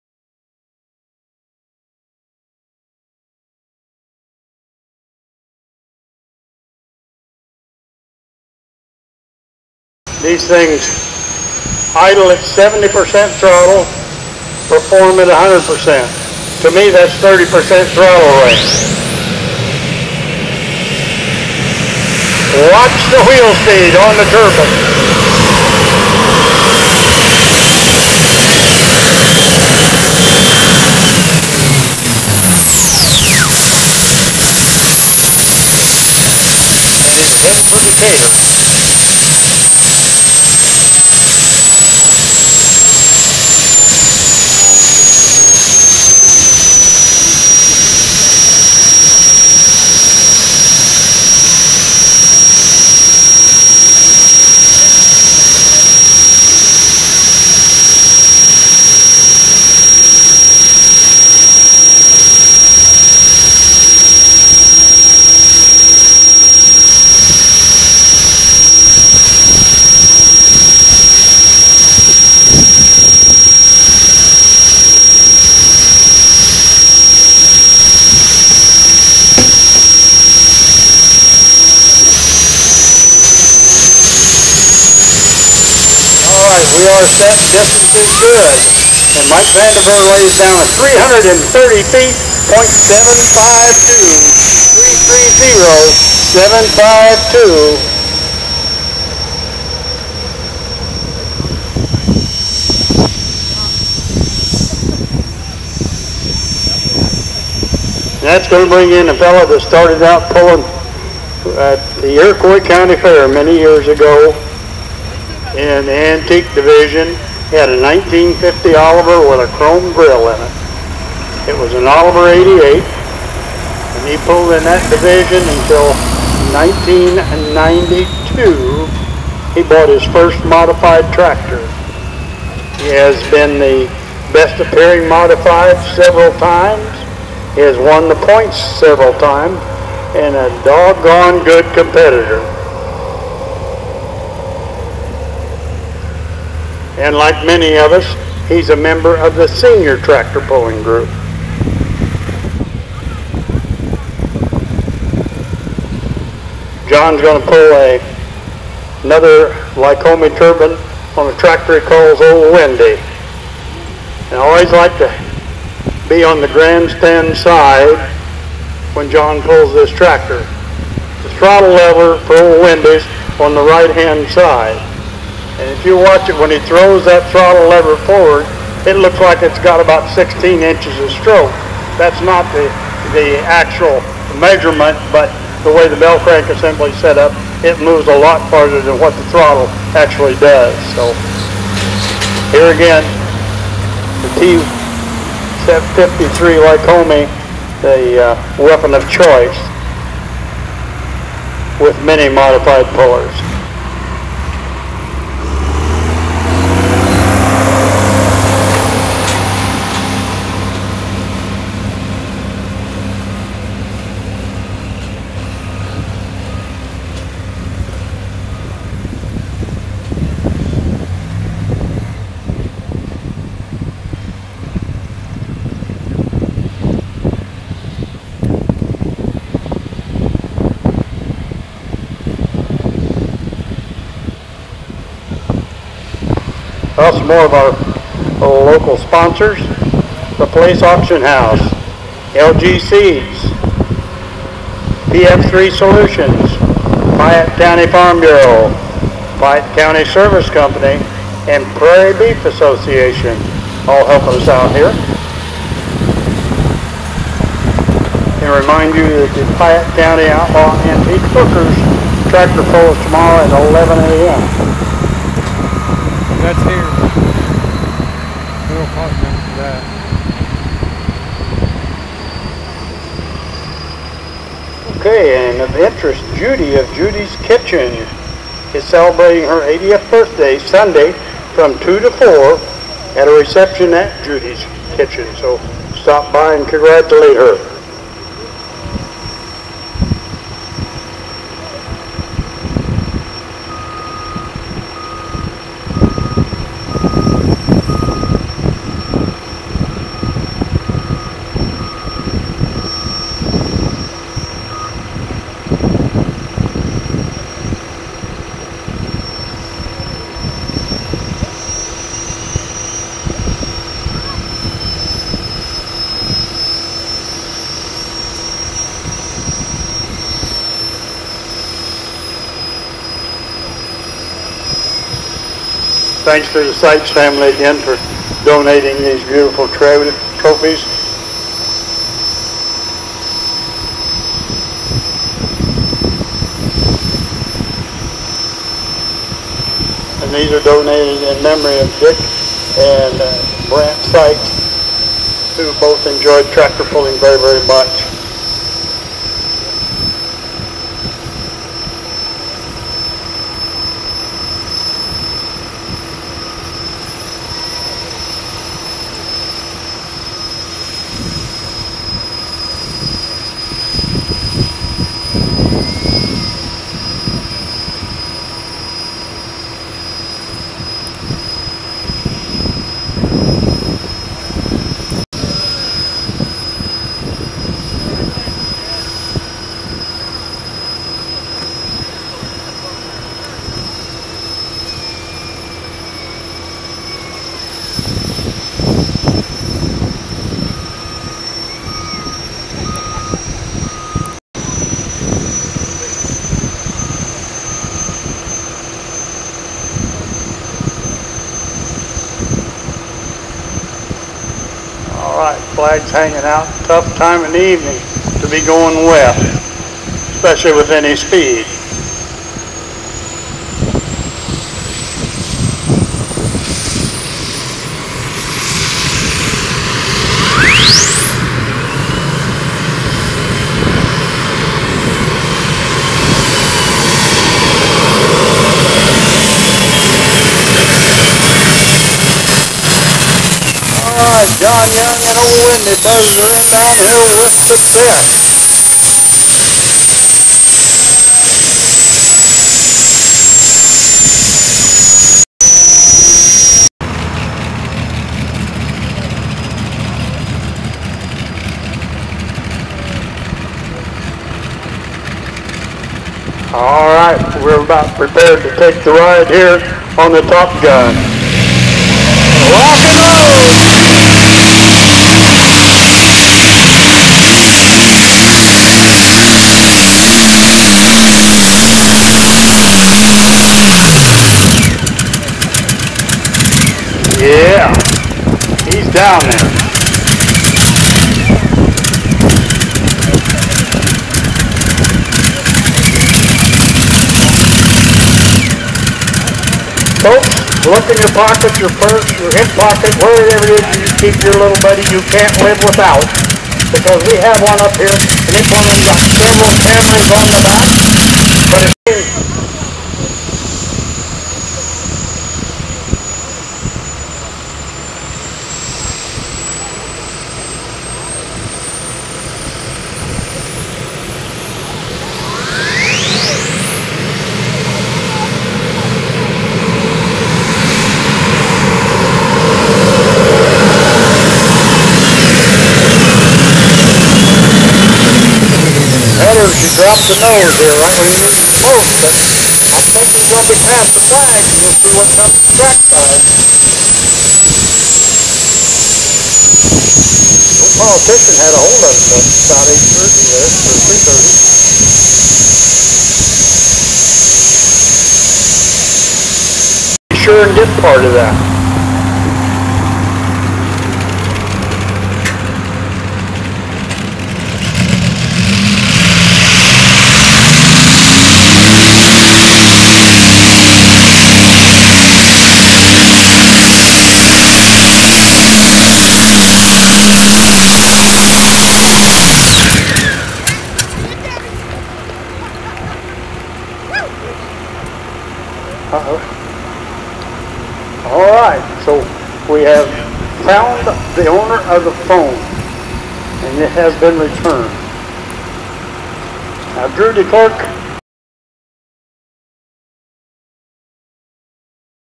Piatt Co Jr Fair – June 15, 2024 Cerro Gordo,Light Modified
1.  Drew DeClerck 338.18
 2.  Curt Micenheimer (Top Gun) 331.02
 3.  Mike Vandeveer 330.75
 4.  John Young 317.08
 5.  Jaron Blomberg    DQ